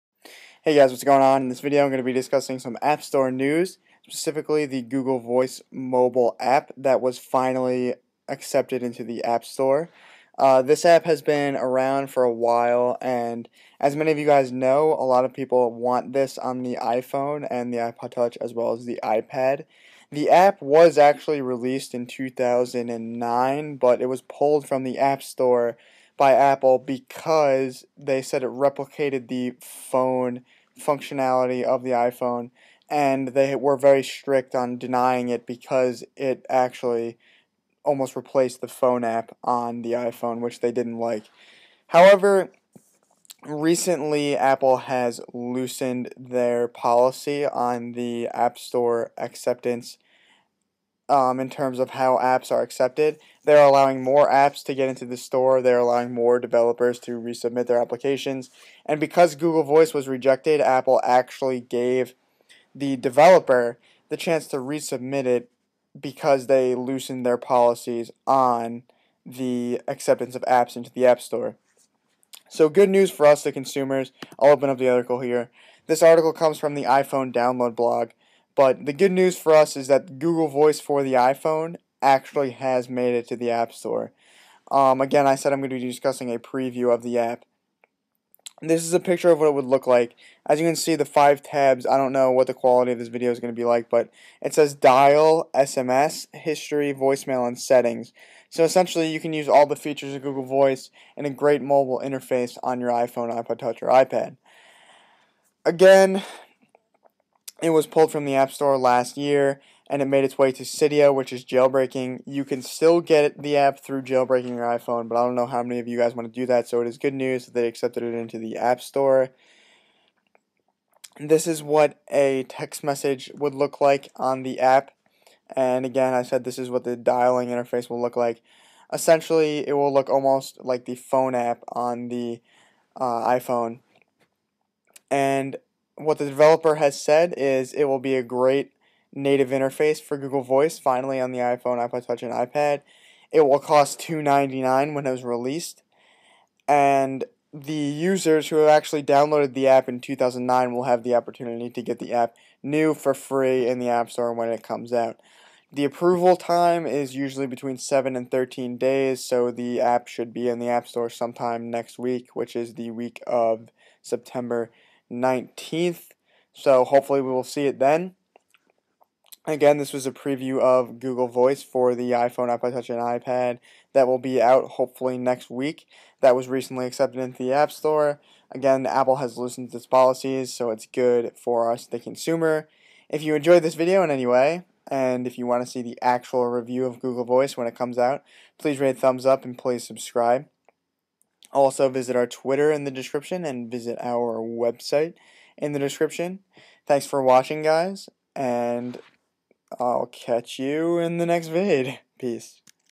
GV Mobile+ : Google Voice Mobile Overview for iPhone, iPod Touch, and iPad
In this video, I overview the Google Voice Mobile App for iPhone, iPod Touch, and iPad. This app was recently released, and many people have been waiting for it.